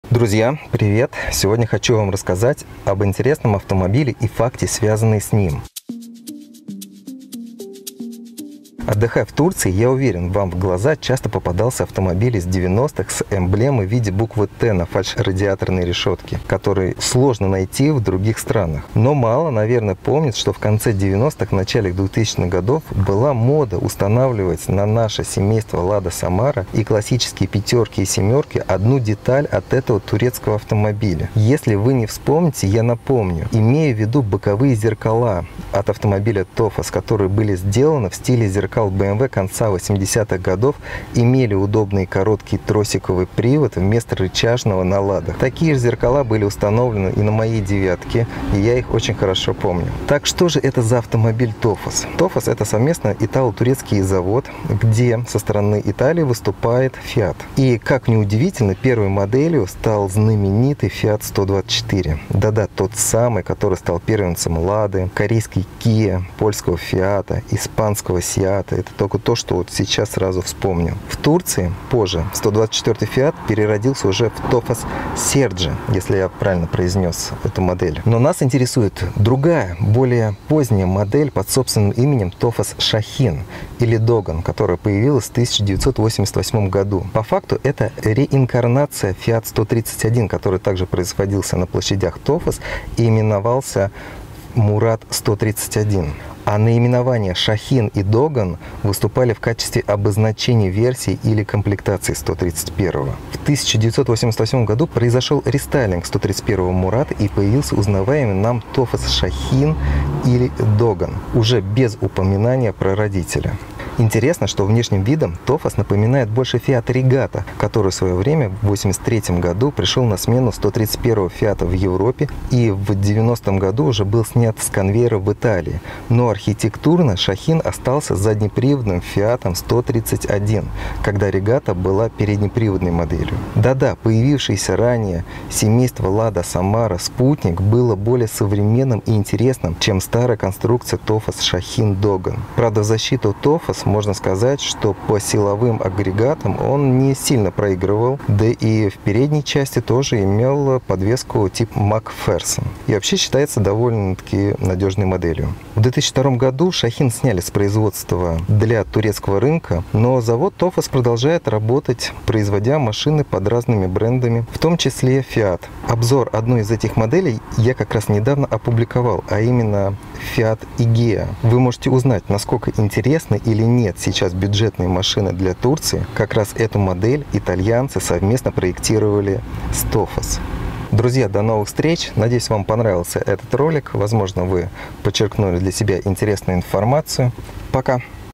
Неизвестный Tofas Sahin & Dogan и интересные факты
Друзья!
Рассматривая разные мировые каталоги автомобилей или приезжая в гости в Турцию, вам скорее всего попадался на глаза неизвестный турецкий автомобиль. Решил вас кратко с ним познакомить, что же это за машина такая и рассказать о интересных фактах связанных с ней, про которые мы забили или даже не догадывались.